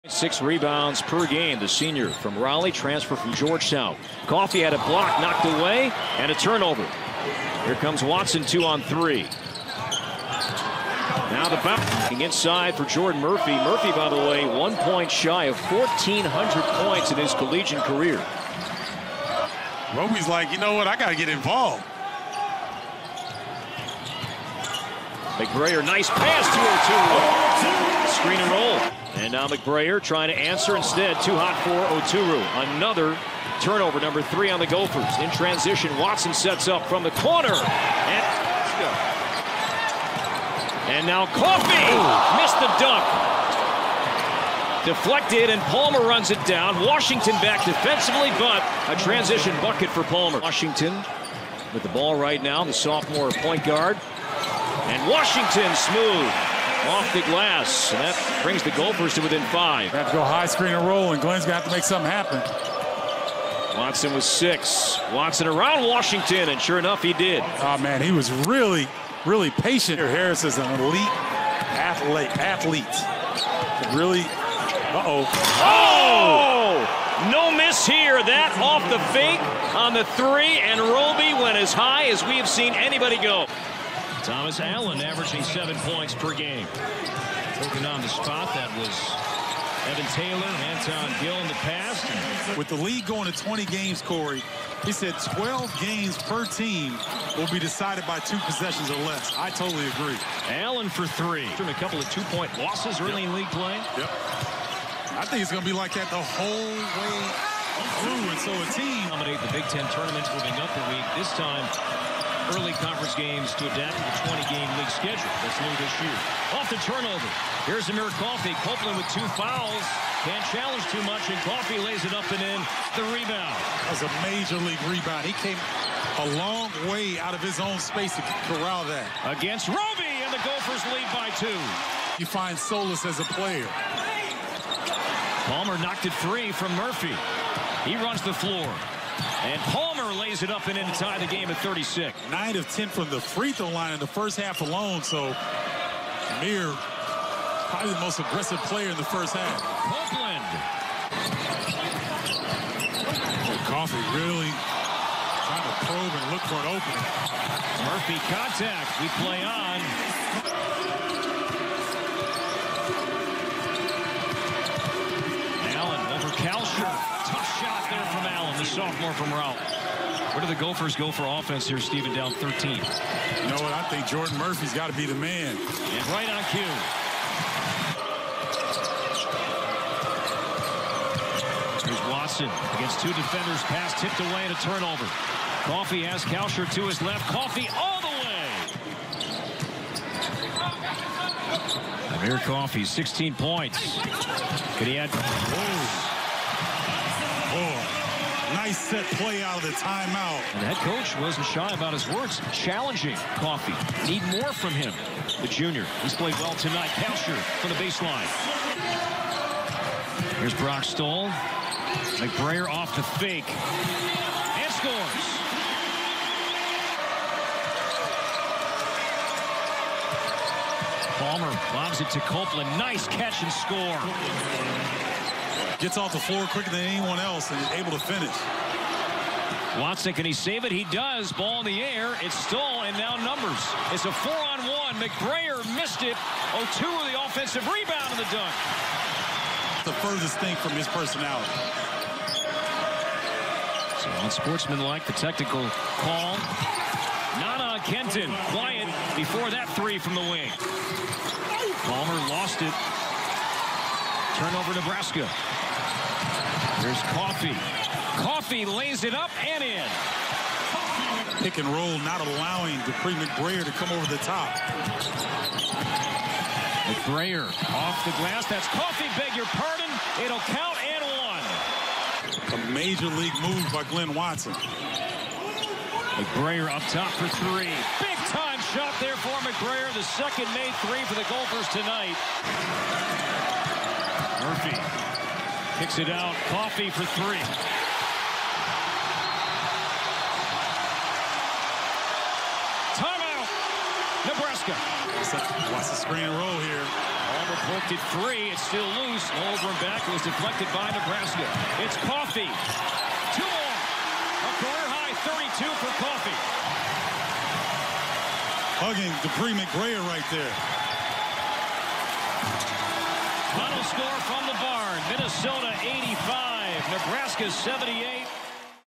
Highlights: Minnesota Golden Gophers vs. Nebraska Cornhuskers | Big Ten Basketball
Amir Coffey scored 32 points to lead Minnesota to a 85-78 win over Nebraska.